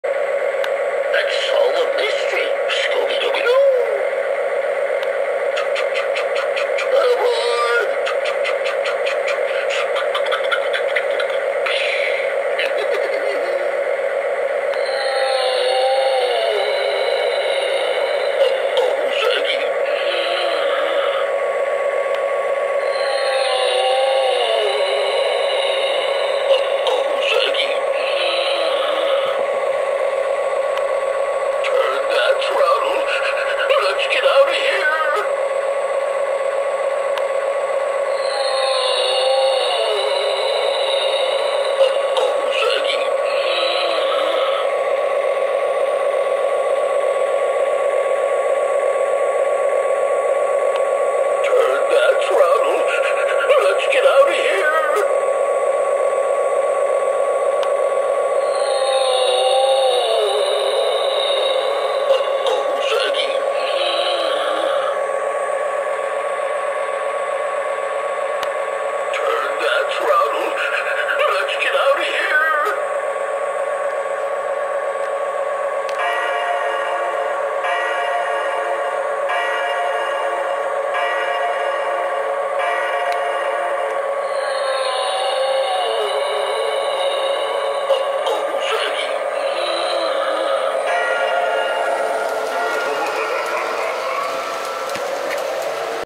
Railchief74 Passenger station announcements challenge
As this train was just set-up on the layout yesterday, it's probably the most appropriate considering the time of the season. Enjoy! Opa Fritz